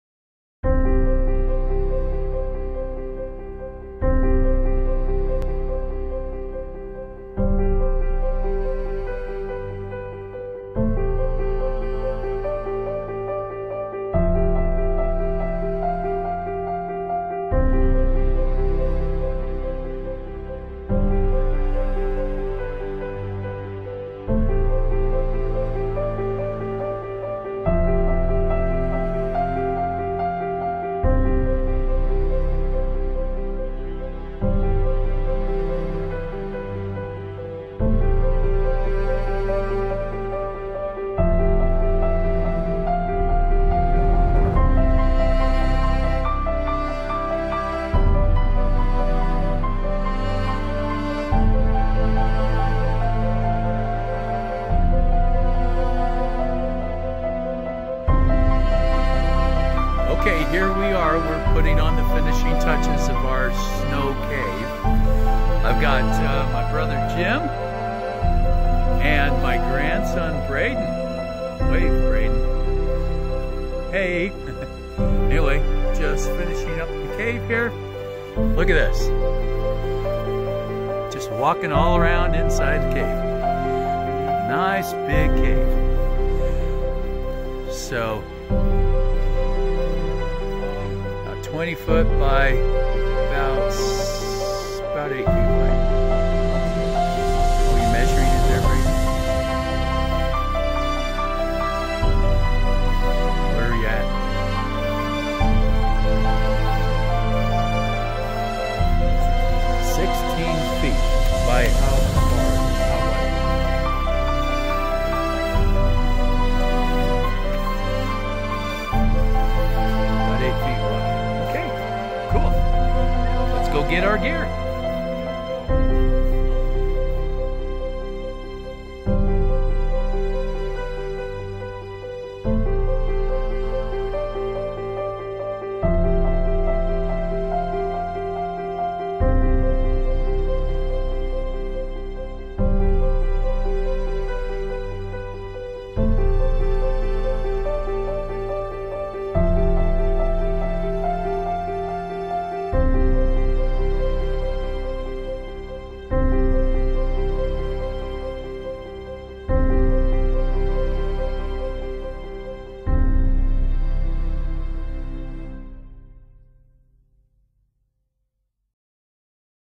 This year's snow caving experience
The tradition lives on...  We have done much larger caves in the past but this one was a very comfortable design that was large enough to sleep up to 8 people.